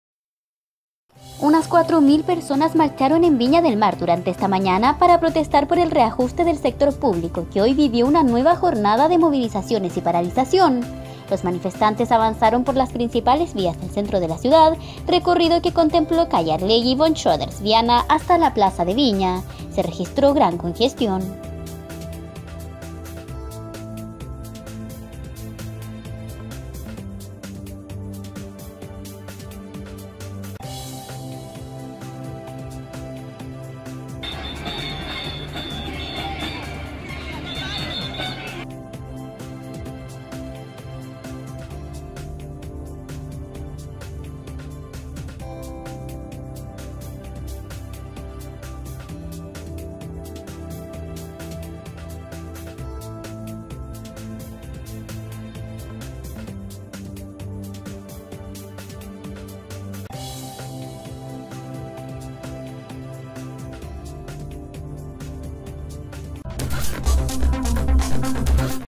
Trabajadores marchan en Viña del Mar en una nueva jornada del paro del sector público
Unas 4 mil personas avanzaron por el centro de la ciudad, pasando por las principales arterias. El recorrido comprendió Arlegui, Von Schröeders, Viana, hasta la Plaza de Viña. Se registró congestión.